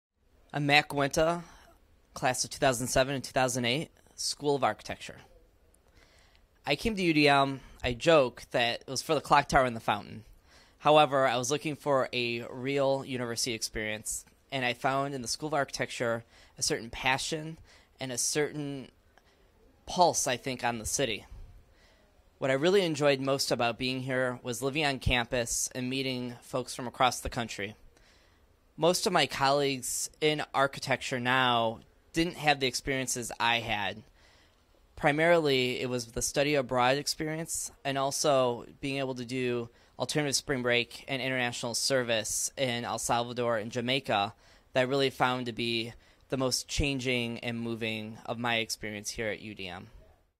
Matt Guinta '07, '08 on what University of Detroit Mercy means to him
University of Detroit Mercy Architecture alumnus Matt Guinta '07, '08 shares some of what the University means to him. He was interviewed at the Detroit Mercy Homecoming Tailgate party, Feb. 13, 2016.

If you've got a story from the University or Mercy College of Detroit, please let us know by visiting